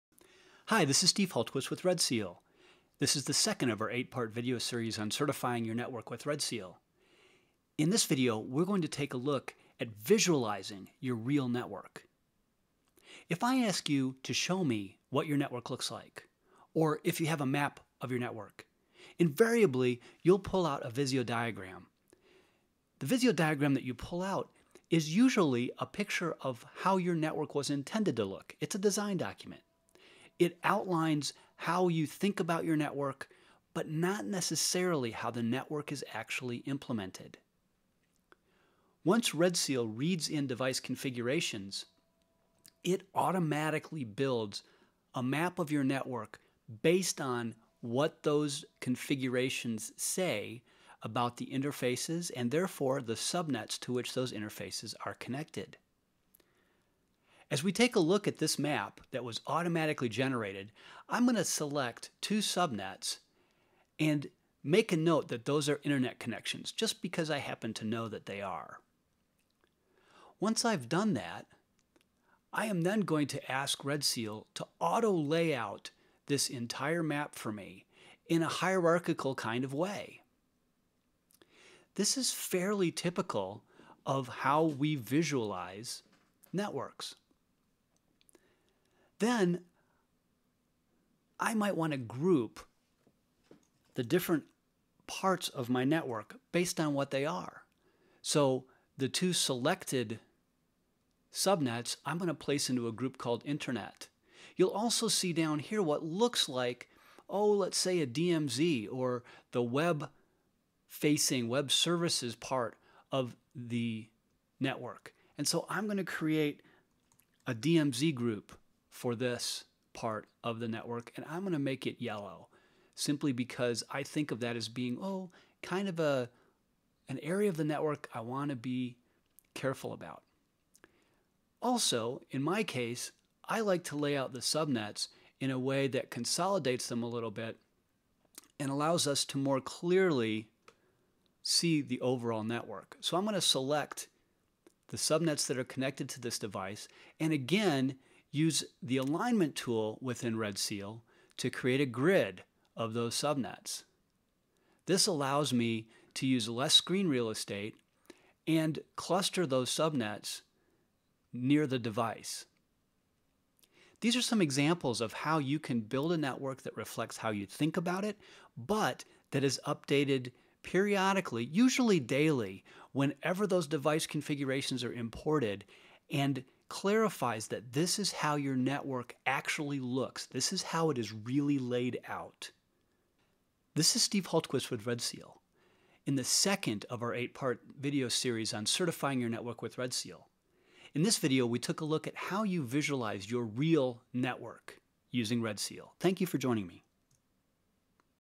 Certifying Your Network with RedSeal - Part 2: Visualizing Your Real Network
Get a closer look at how RedSeal creates an accurate, up-to-date map of your network.